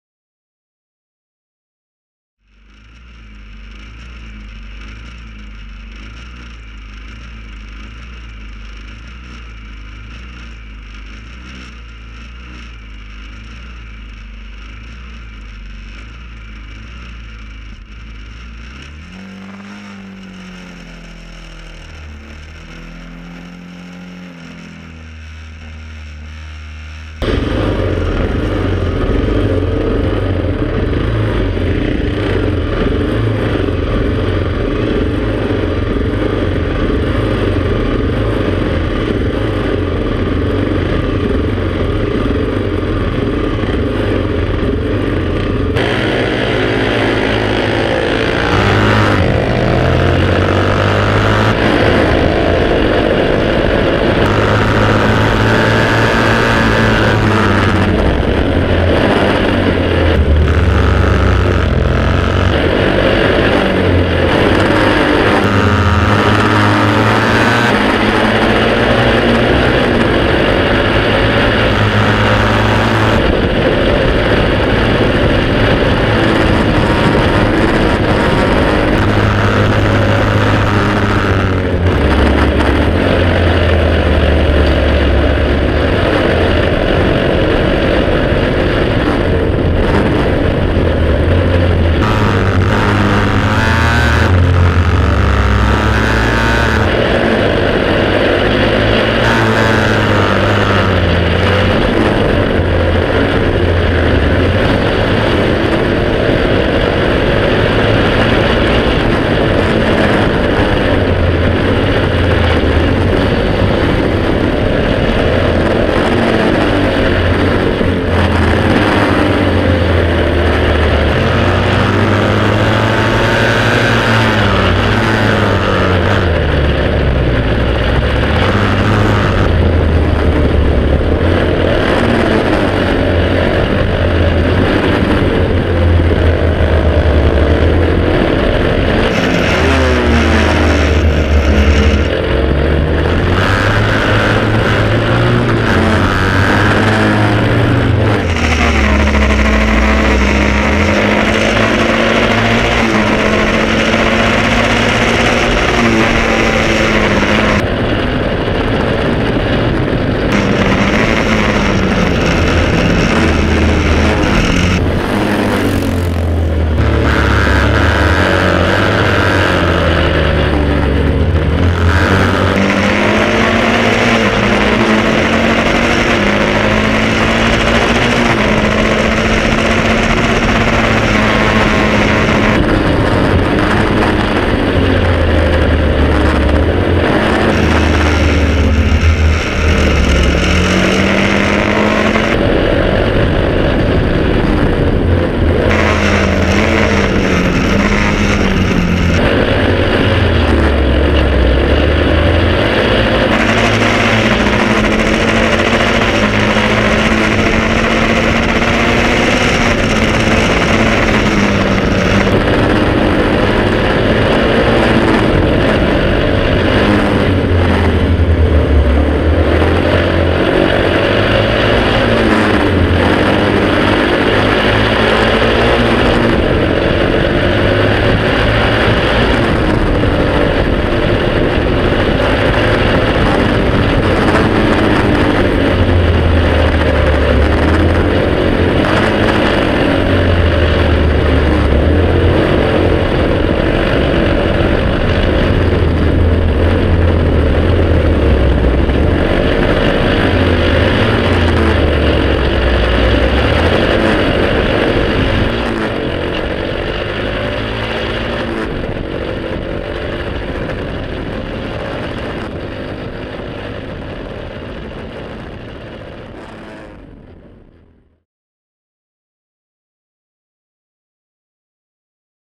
Velocettes Racing 1st, 2nd, 3rd, 6th Burt Munro Street Race 2015
On bike real time footage of the fastest prewar street racing KTT Velocettes anywhere in the world.